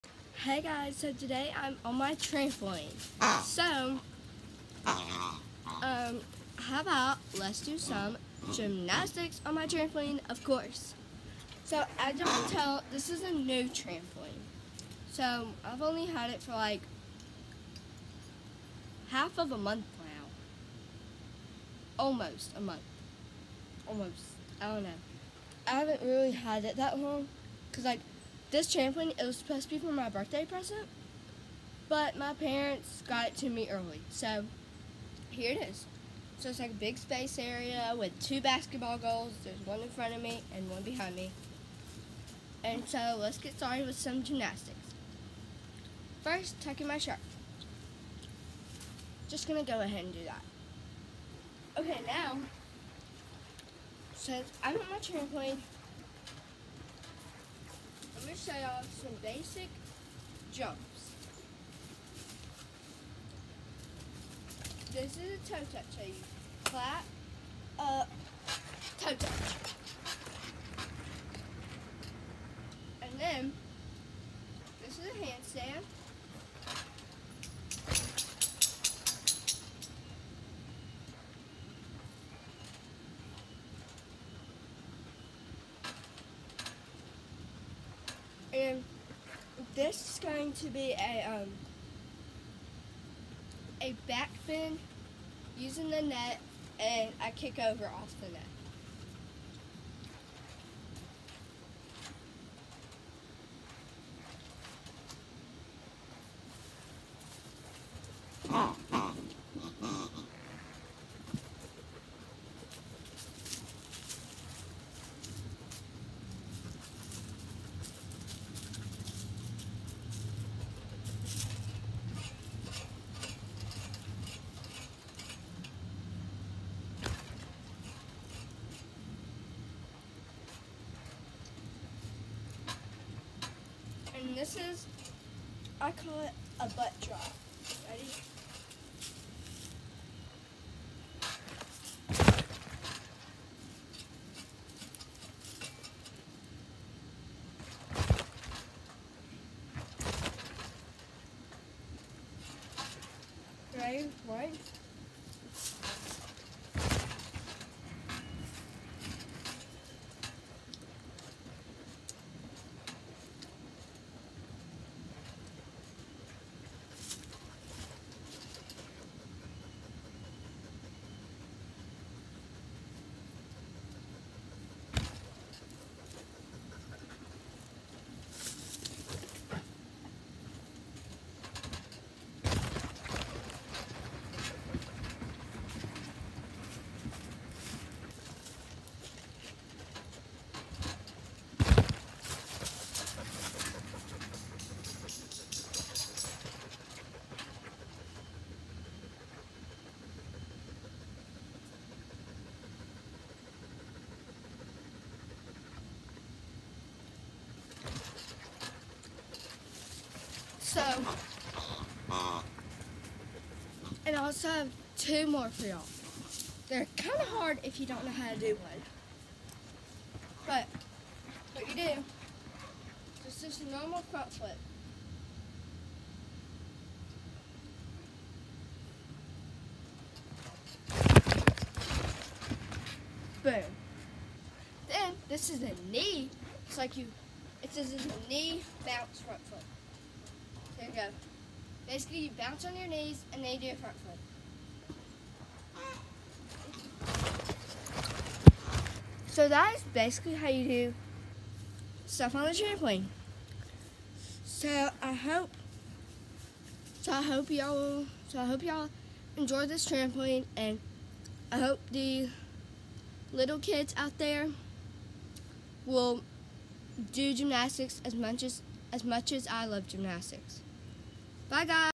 EMMA’S gymnastics on her trampoline
So today i am going to be doing gymnastics on my trampoline and I will be showing y’all some of my tricks on it and some other things